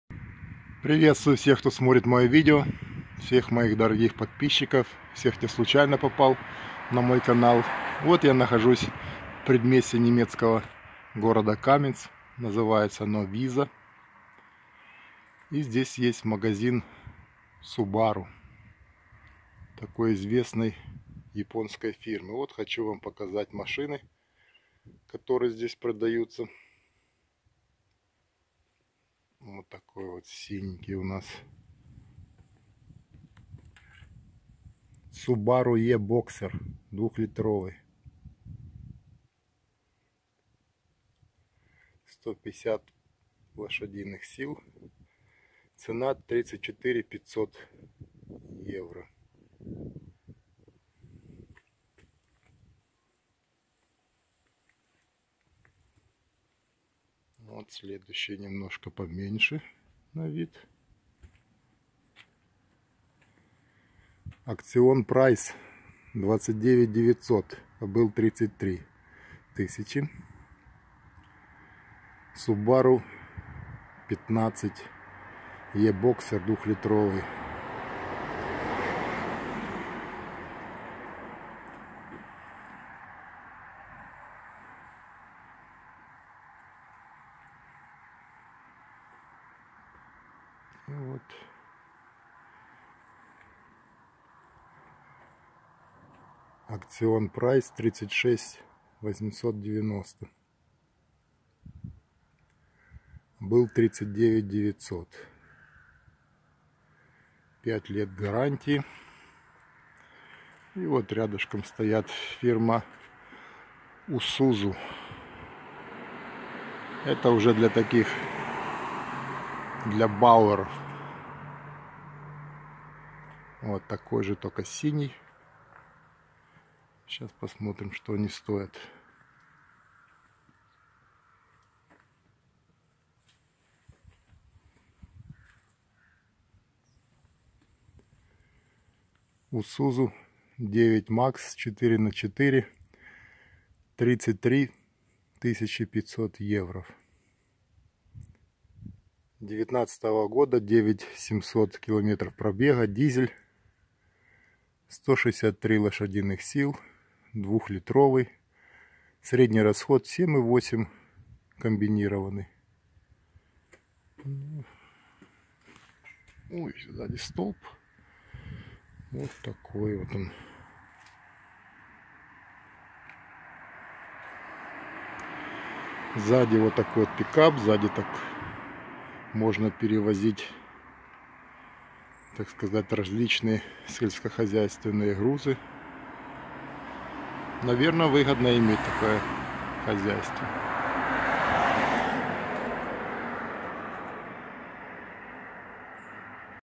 КаменцГермания Цены на авто в автомагазине Autohaus Löhnert Subaru/Isuzu. Каменц. Виза (Wiesa).
Цены на авто в автомагазине Autohaus Löhnert Subaru/Isuzu. Каменц. Виза (Wiesa).
- мой плейлист о магазинах немецкого города Каменц.